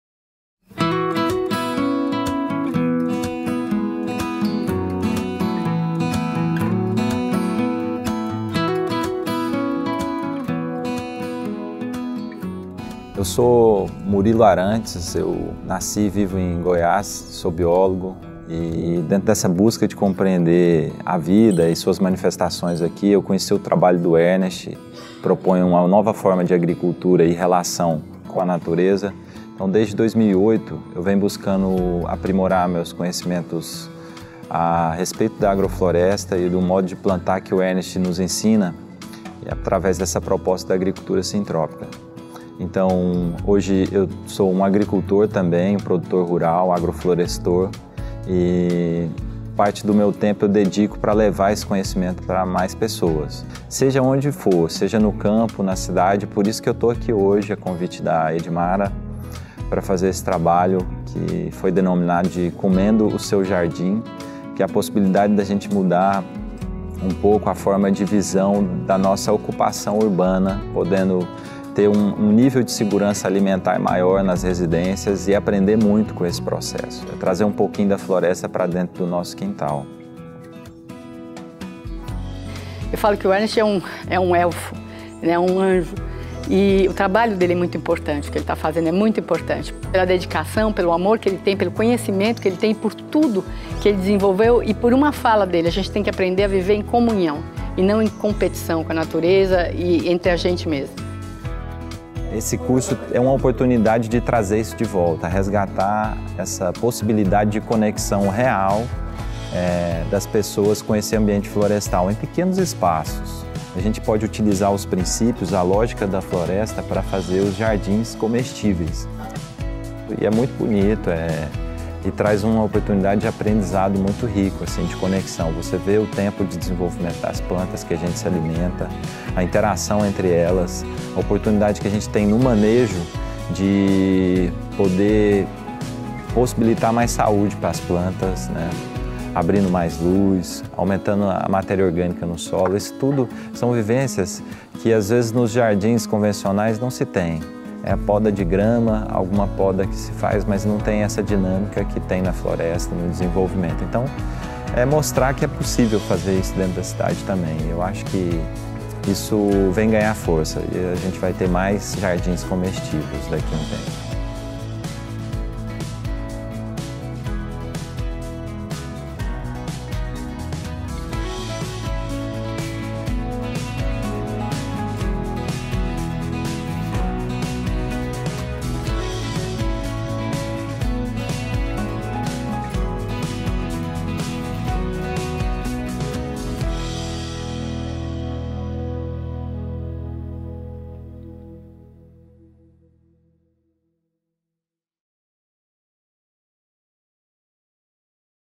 Jardins Comestíveis - Agricultura Urbana - Agrofloresta na Cidade
Por iniciativa de Edmara Barbosa, dentro do projeto Nós Fazemos o Clima ), o vídeo mostra um pouco do Curso de Agricultura Urbana Agroflorestal realizado em fevereiro de 2019 na cidade de São Paulo. O curso foi ministrado pelo instrutor Murilo Arantes da Agrosintropia A experiência desta iniciativa mostra que podemos aplicar boa parte dos princípios da Agricultura Sintrópica em pequenos espaços urbanos.